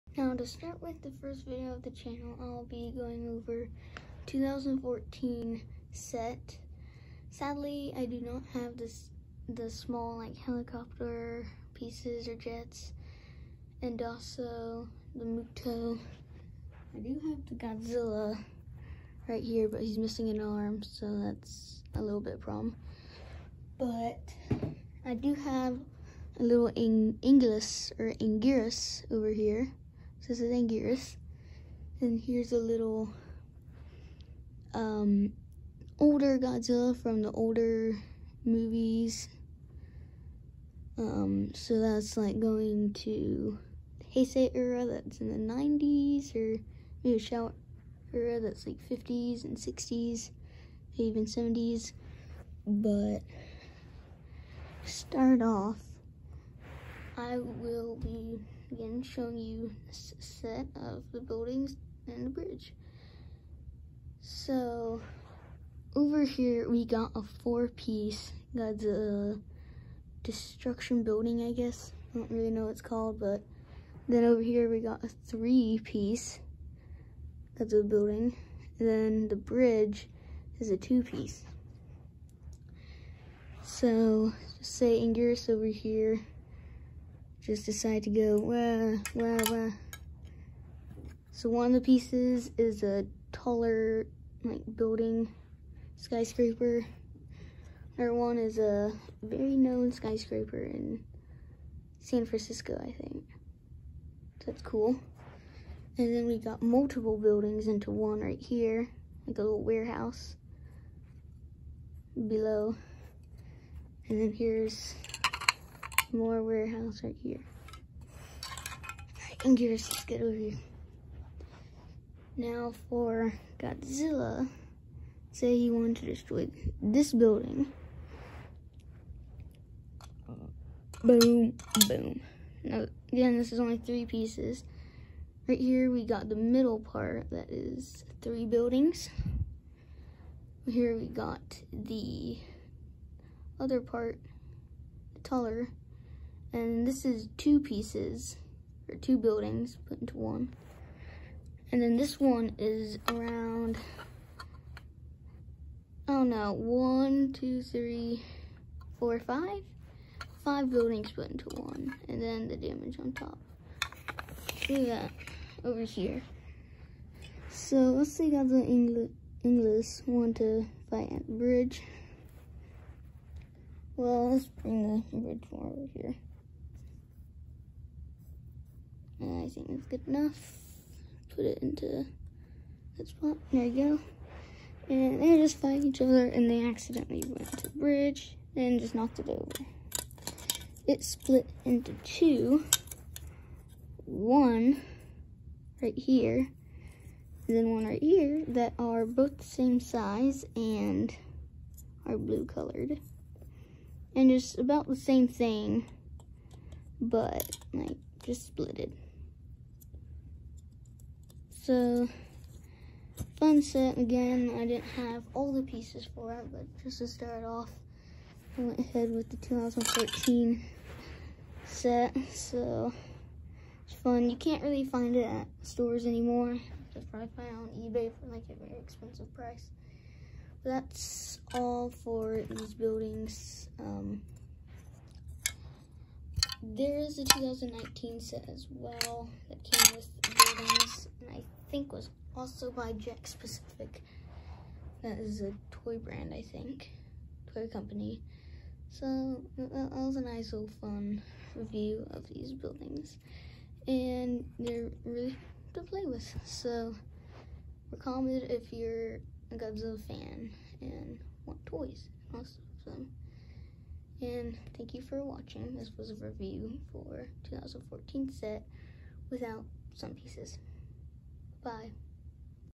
Godzilla (2014) Destruction City Playset Toy Review!
This is the first video on the channel, today, we’re going over the 2014, Godzilla Destruction City Playset, although I’m missing pieces, it’s still a very fun playset!